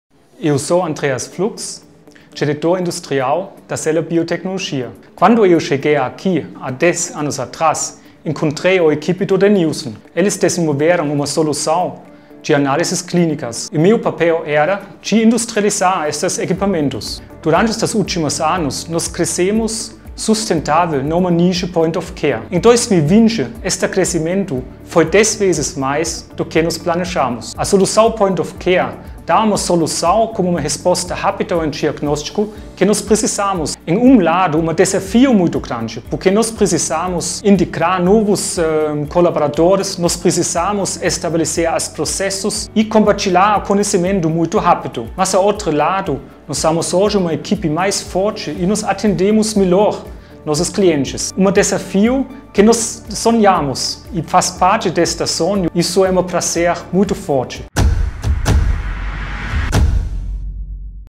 Conheça um pouco mais da nossa história
O nosso Diretor Industrial, Andreas Flugs, chegou a Celer há mais de dez anos com o objetivo de industrializar soluções em análises clínicas. Veja quais são os desafios enfrentados e as conquistas!